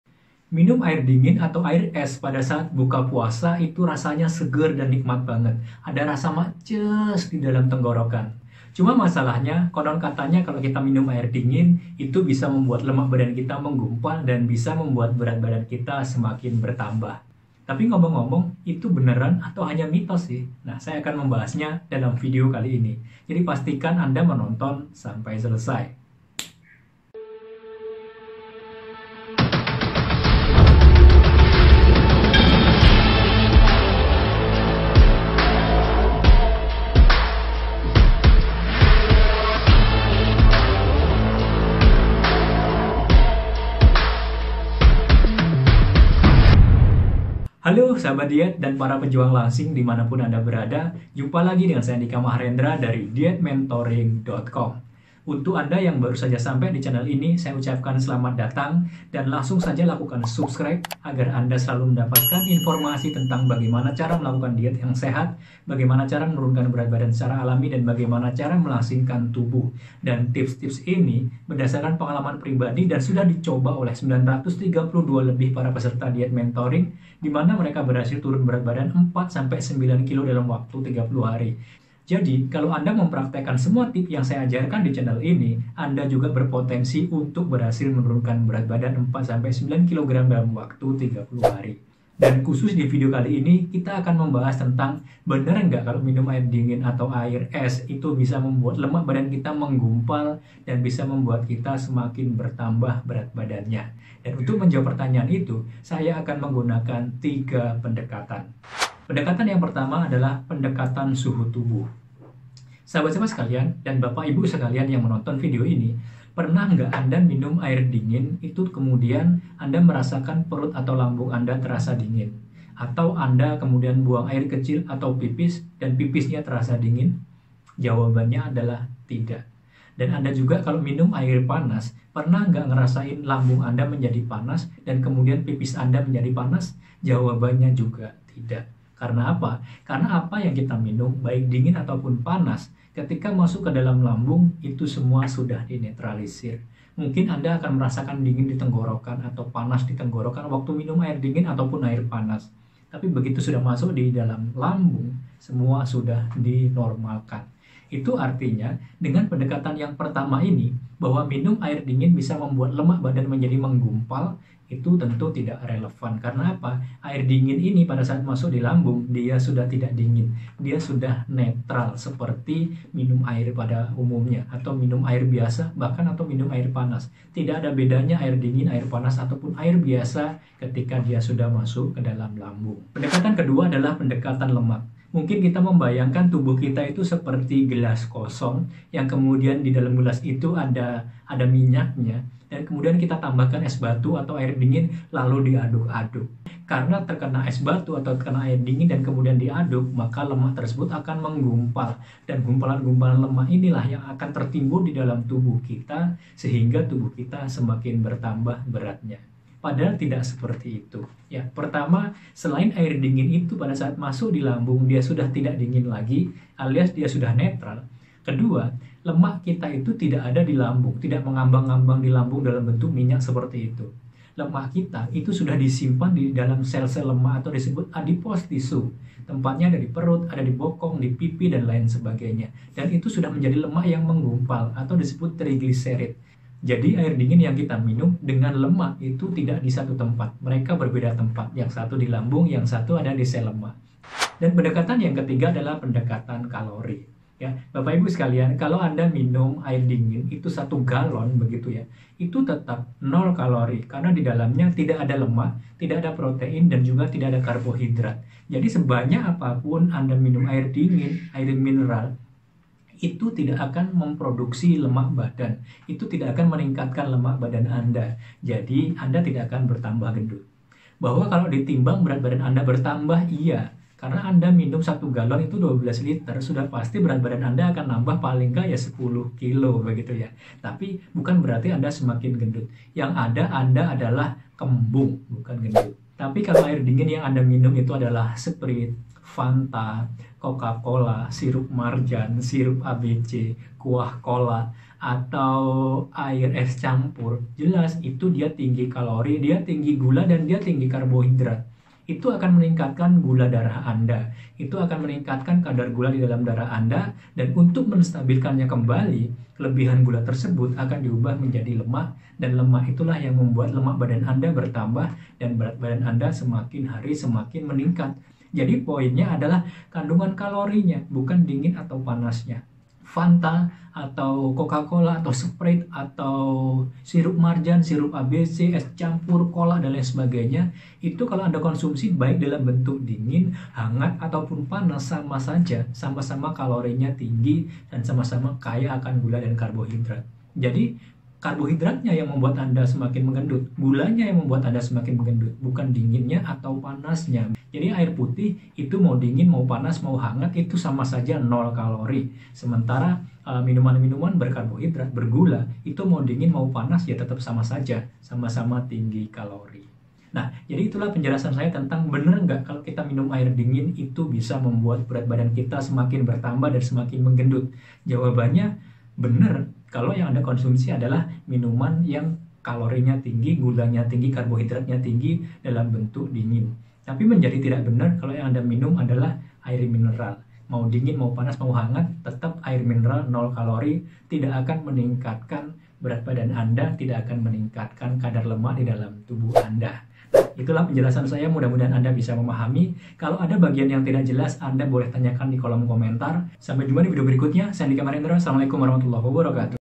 Minum Air Dingin Saat Berbuka Bisa Bikin Gemuk, Benarkah? - Ndika Mahrendra
Tips Diet kita kali ini adalah tentang Apakah Minum Air Dingin Bisa Membuat Gemuk. Itu beneran atau cuma mitos ya.